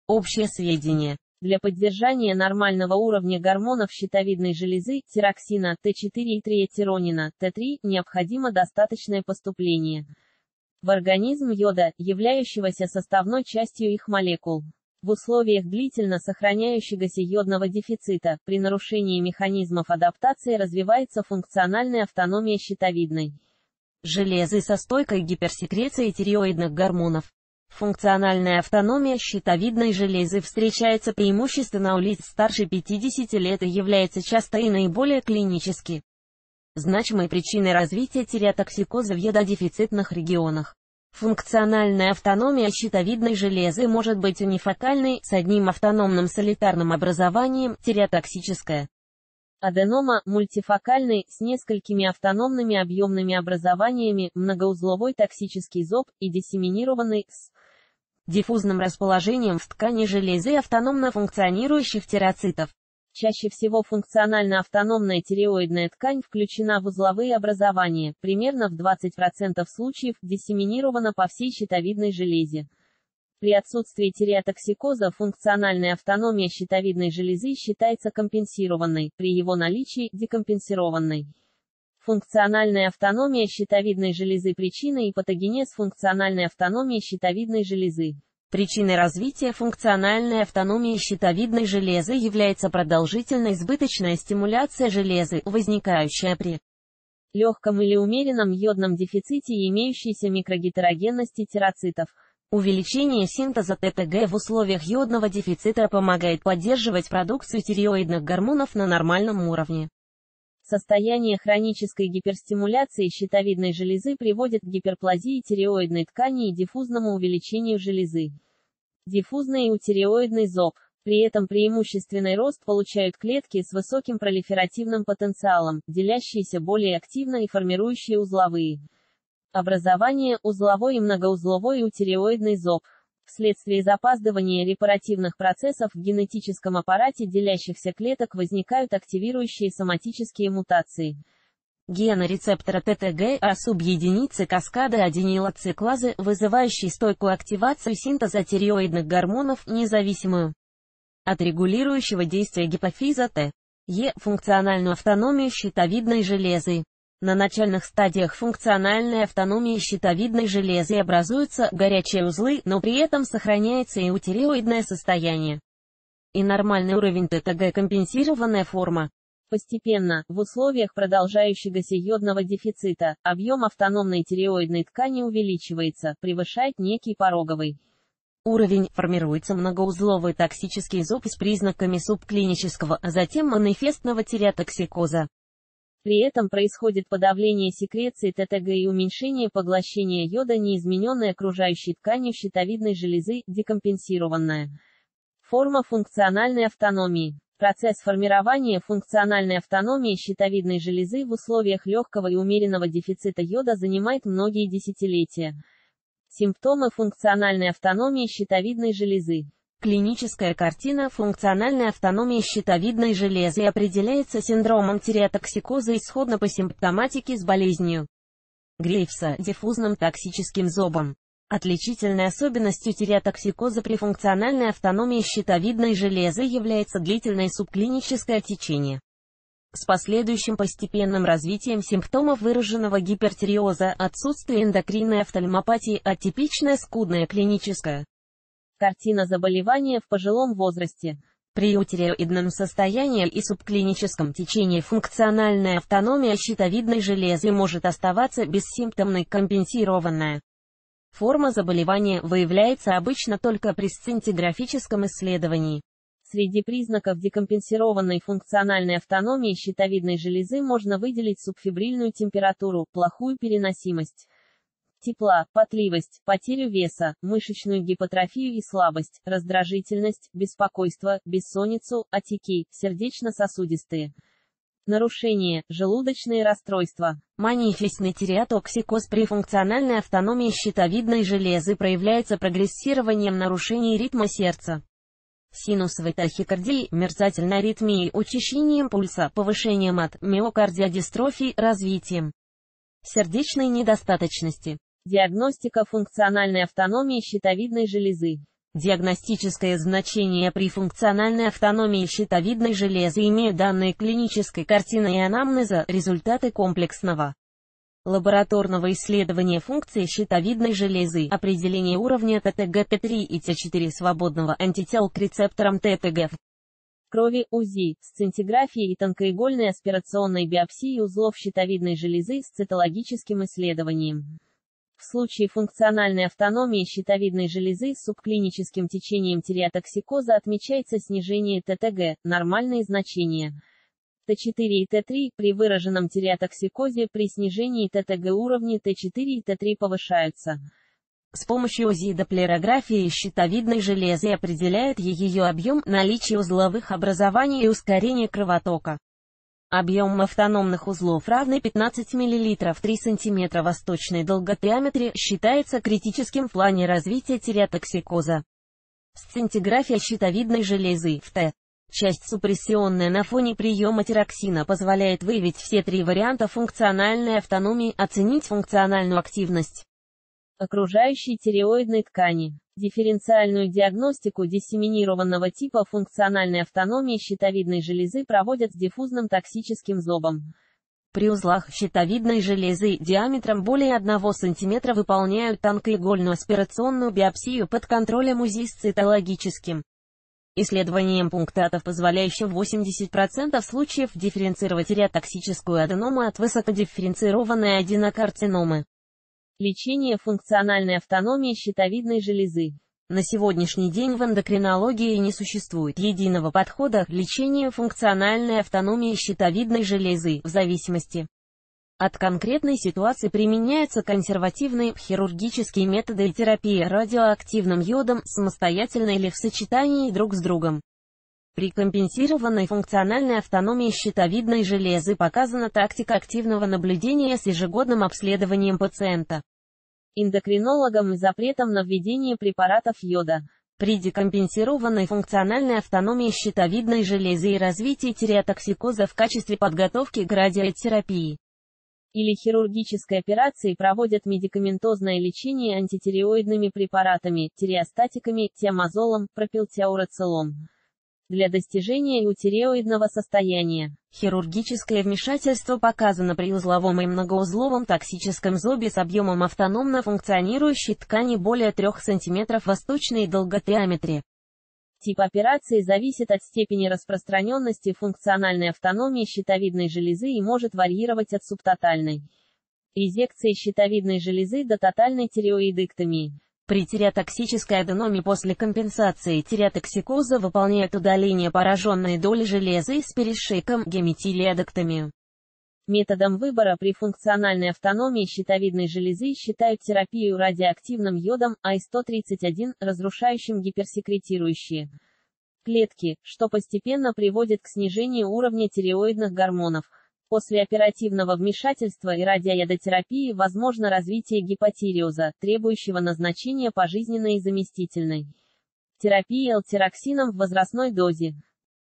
Функциональная автономия щитовидной железы
Функциональная автономия щитовидной железы. Симптомы. Диагностика. Что делать при диагнозе функциональная автономия щитовидной железы. Консервативное лечение и операции. Платные и бесплатные клиники, в которых лечится функциональная автономия щитовидной железы.